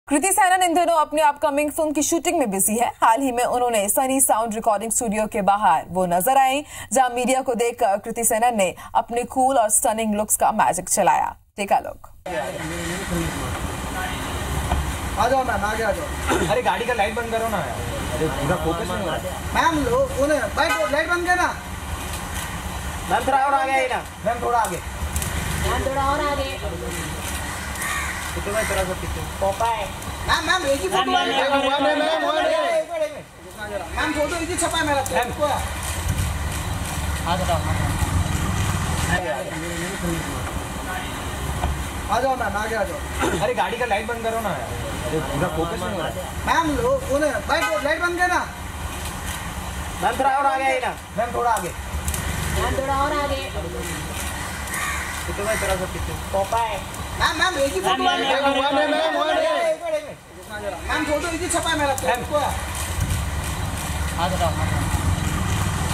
Recording करने पहुंची Kriti Sanon | Media को दिया कृति ने Pose
IBC24, Recording करने पहुंची Kriti Sanon, Media को दिया कृति ने Pose, Kriti Sanon

Incoming Search Terms: